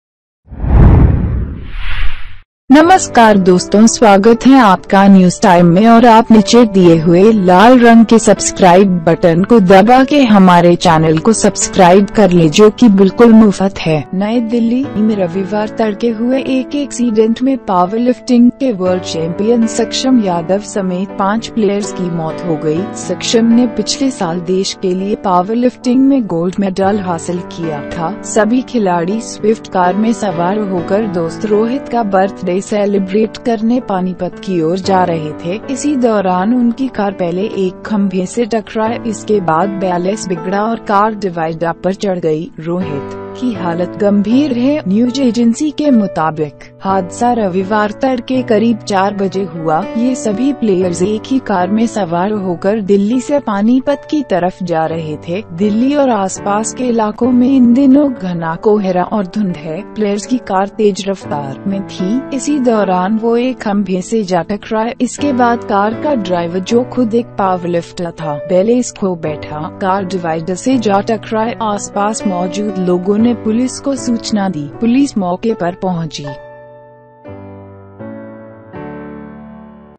वर्ल्ड Powerlifting चैम्पियन की हादसे में मौत, दोस्त का बर्थडे सेलिब्रेट करने जा रहे थे
Four Powerlifting Players
New Delhi News
New Delhi.In the Sunday morning, five players, including World Champion enabled Yadav, were killed in an accident. The competent had achieved Gold Medal in the powerlifting for the country last year. All the players were heading towards the Swift car and going to Panipat to celebrate friend Rohit's birthday. During this time his car collided with a pillar first. After this the balance went up and the car dividers climbed. Rohit's condition is serious. According to the News Agency, the incident occurred at around four o'clock in the morning. All these players were riding in a car and going from Delhi to Panipat. Dense fog and fog in Delhi and the surrounding areas these days. Players' car was at a fast pace. During this time he collided with a pillar.
After this the driver of the car who was himself a powerlifter, lost the balance. Going from car dividers collides. People around, informed the police. Police arrived on the spot
नई दिल्ली.राजधानी में रविवार तड़के हुए एक एक्सीडेंट में पावरलिफ्टिंग के वर्ल्ड चैम्पियन सक्षम यादव समेत 5 प्लेयर्स की मौत हो गई। सक्षम ने पिछले साल देश के लिए पावरलिफ्टिंग में गोल्ड मैडल हासिल किया था। सभी खिलाड़ी स्विफ्ट कार में सवार होकर दोस्त रोहित का बर्थडे सेलिब्रेट करने पानीपत की ओर जा रहे थे। इसी दौरान उनकी कार पहले एक खंभे से टकराई। इसके बाद बैलेंस बिगड़ा और कार डिवाइडर पर चढ़ गई। रोहित की हालत गंभीर है।न्यूज एजेंसी के मुताबिक, हादसा रविवार तड़के करीब चार बजे हुआ। ये सभी प्लेयर्स एक ही कार में सवार होकर दिल्ली से पानीपत की तरफ जा रहे थे। दिल्ली और आसपास के इलाकों मे इन दिनों घना कोहरा और धुंध है। प्लेयर्स की कार तेज रफ्तार में थी। इसी दौरान वो एक खंभे से जा टकराई।
इसके बाद कार का ड्राइवर जो खुद एक पावरलिफ्टर था, बैलेंस खो बैठा। कार डिवाइडर से जा टकराई। आसपास मौजूद लोगों ने पुलिस को सूचना दी। पुलिस मौके पर पहुंची